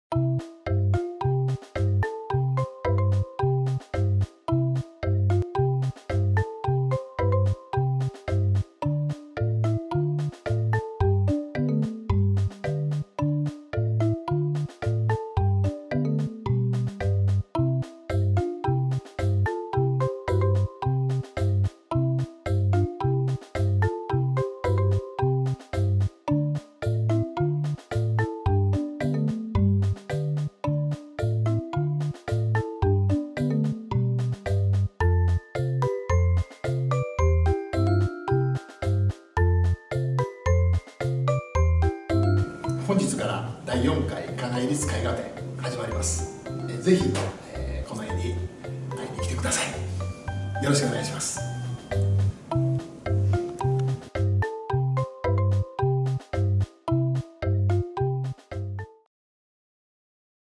【終了】第4回 金井 立 絵画展 自然・JINEN
主催: 金井 立 Ritsu Kanai
イーグレひめじ 地下1階 市民ギャラリー
兵庫県姫路市本町68-290
8/2㈪～8(日)10:00～17:00

大阪・兵庫・奈良のお教室のお問合せ等は、

「アクリルガッシュで風景画」を配信していきます。
ご質問は、メール、コメントでお願いします。